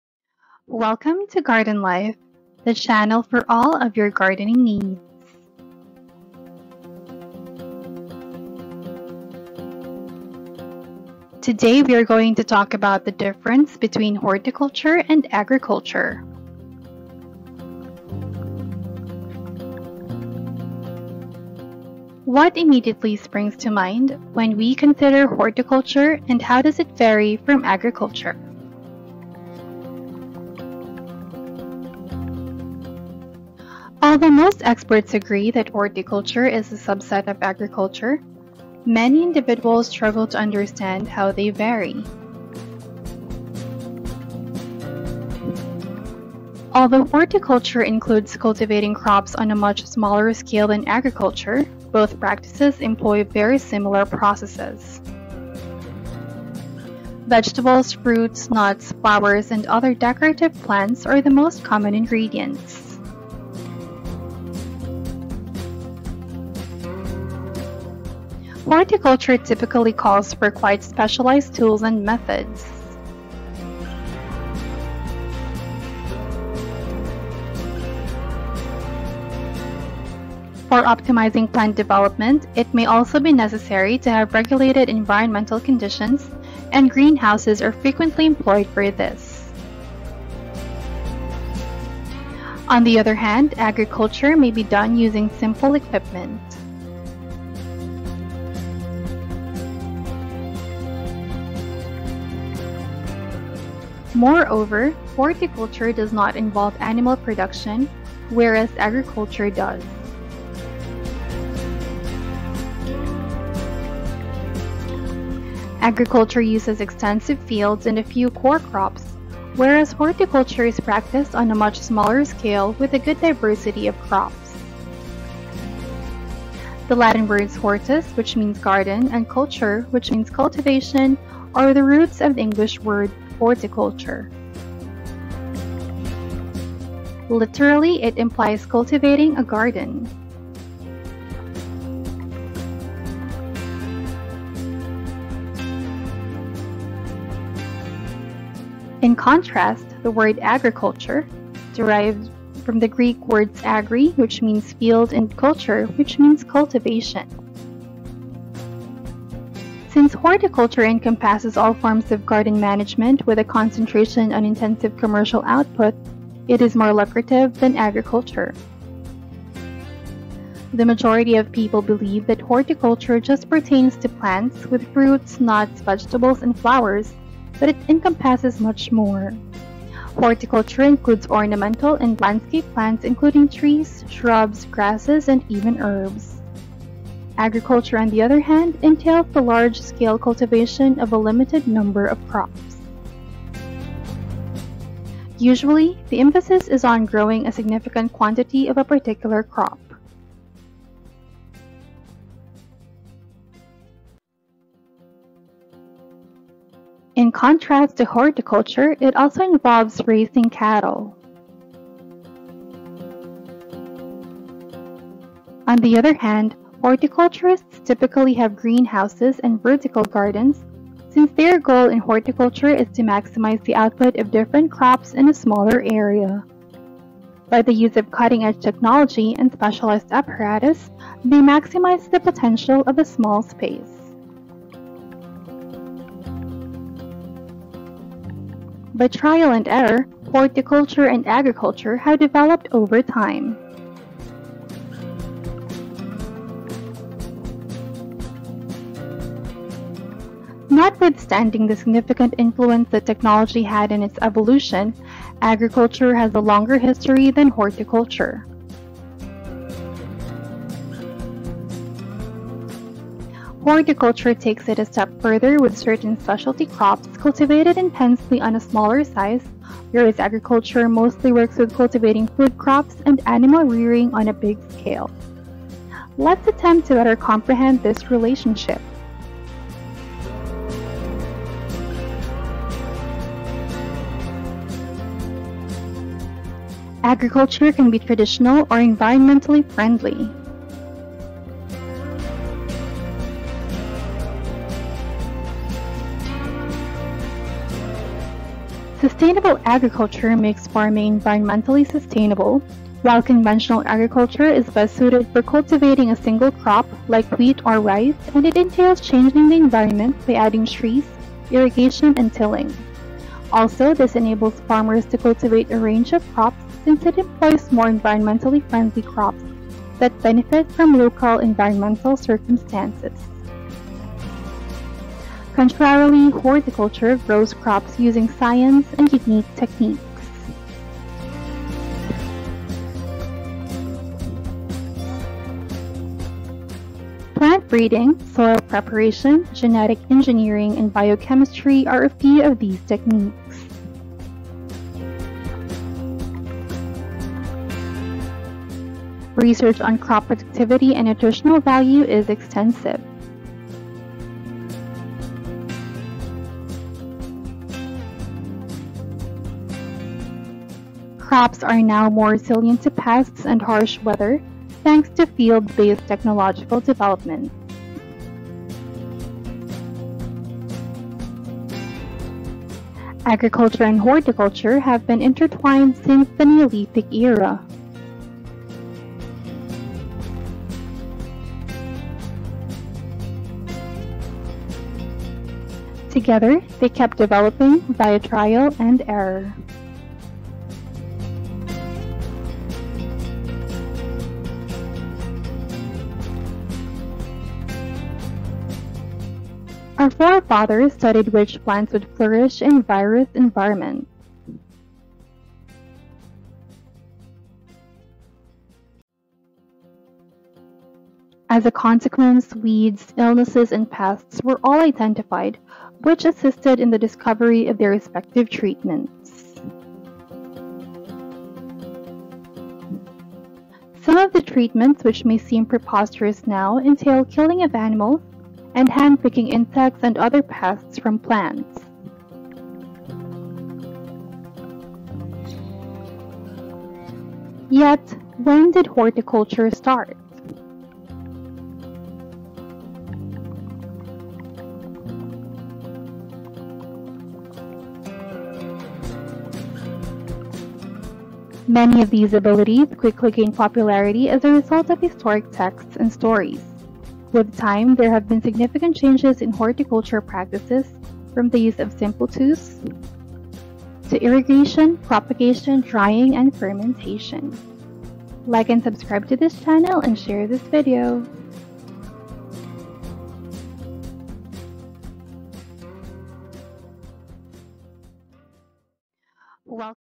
Horticulture vs. Agriculture: You Won't Believe the Surprising Secrets We Unearth!
🌱🚜 Dive into the fascinating world of horticulture and agriculture with us as we uncover the surprising secrets that set them apart! In this eye-opening video, we'll explore the key differences between these two essential practices, their unique techniques, and the incredible impact they have on our daily lives. From the lush gardens of horticulture to the vast fields of agriculture, we'll unearth the hidden gems that make each practice special. Join us on this captivating journey and discover the amazing world of plants and food production like never before! 🌿🌾

mindset -Blowing Differences Between Horticulture and Agriculture
difference between agriculture and horticulture
bsc agriculture